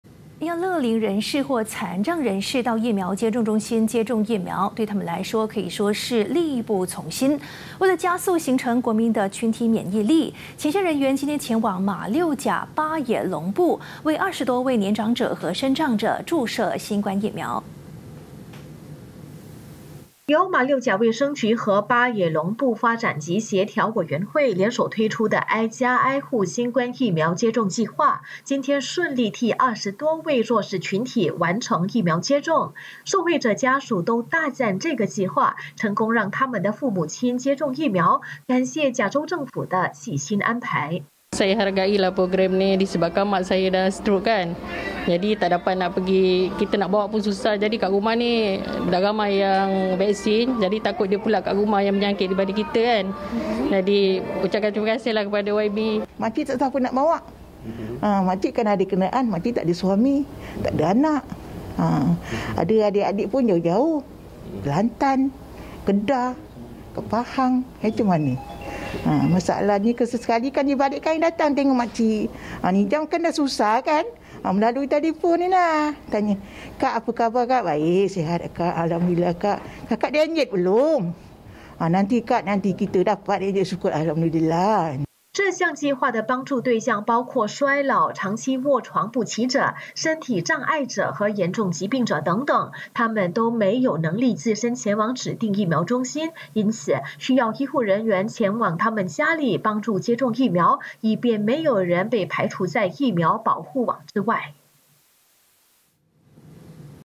【古城挨家挨户接种疫苗 巴也隆布乐龄身障者受惠】
要乐龄人士或残障人士到疫苗接种中心接种疫苗对他们来说可说是力不从心。

为了加速形成国民的群体免疫力，前线人员今天前往马六甲巴也隆布，为20多位年长者和身障者注射新冠疫苗。